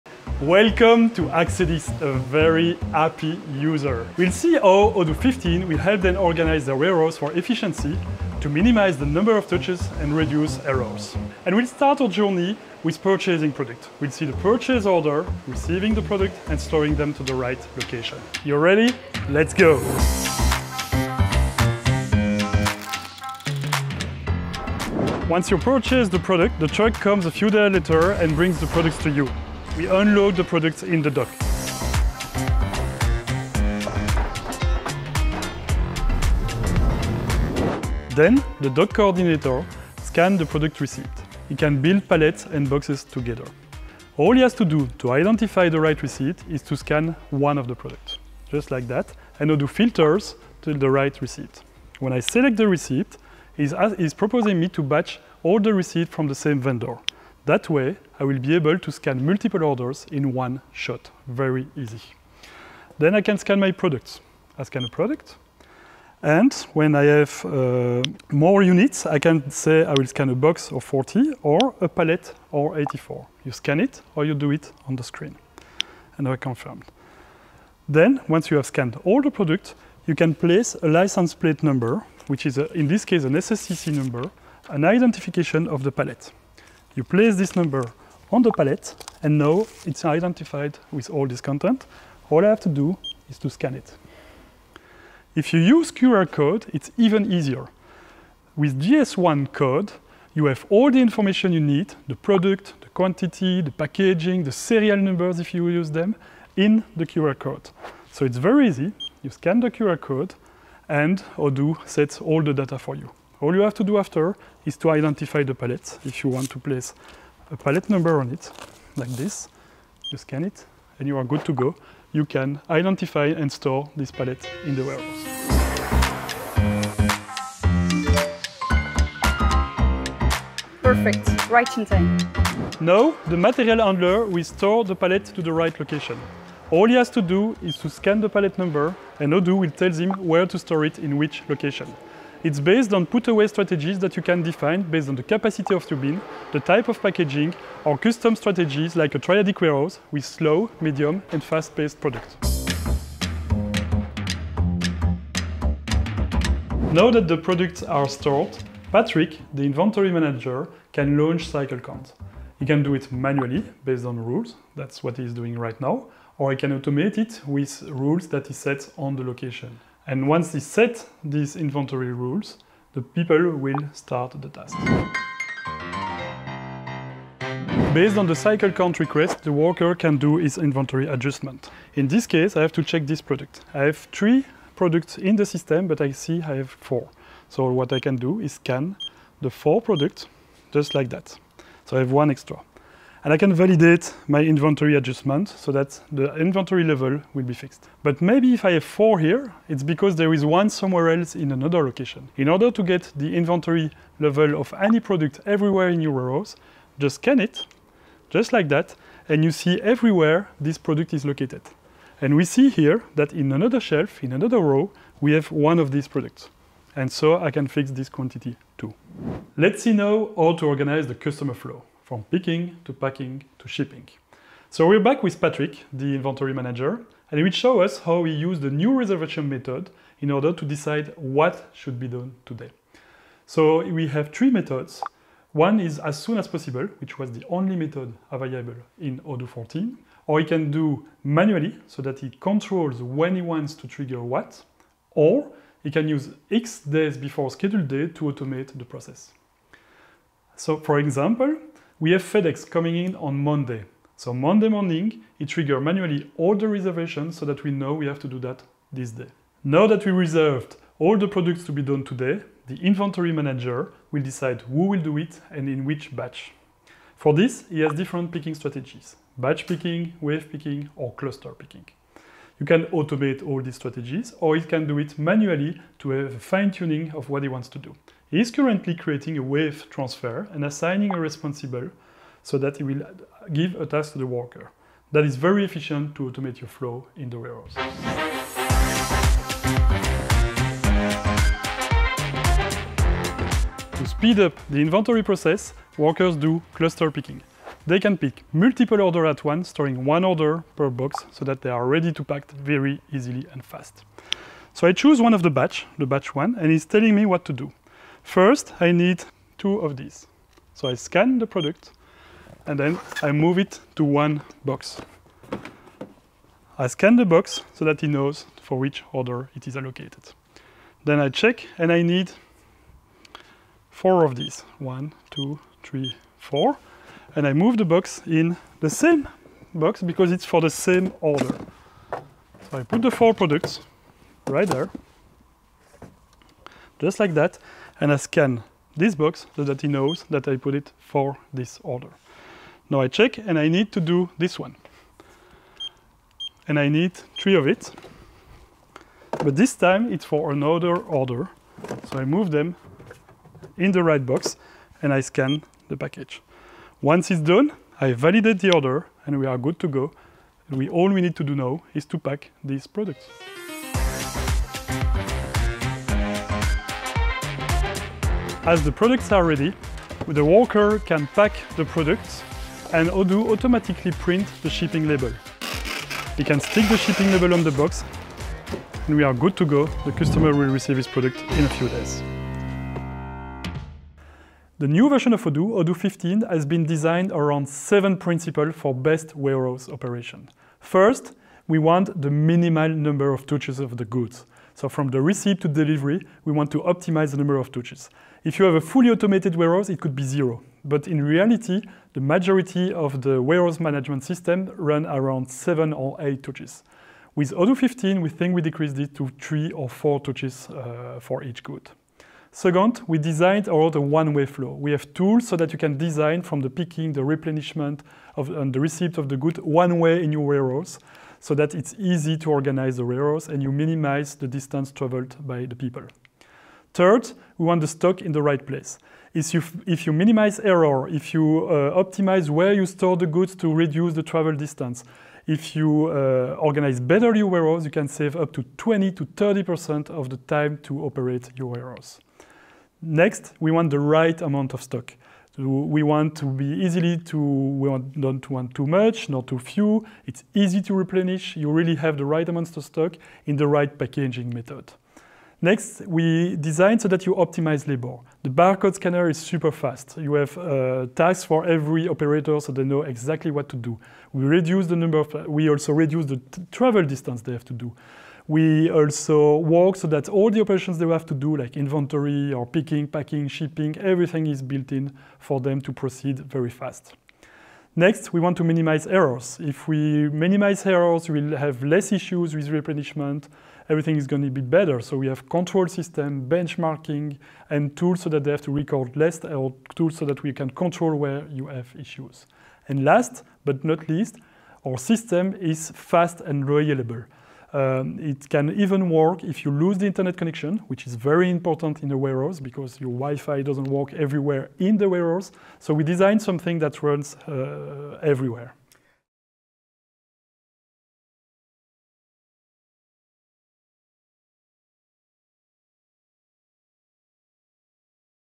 Efficient warehouse management with barcodes - Odoo Inventory
From receiving pallets of goods to sending parcels to customers, follow Fabien in his complete walk-through of Odoo's Warehouse Management System (WMS) at Axedis.

This video was first presented during the 2021 keynote "Unveiling Odoo 15: The Best Keynote Ever"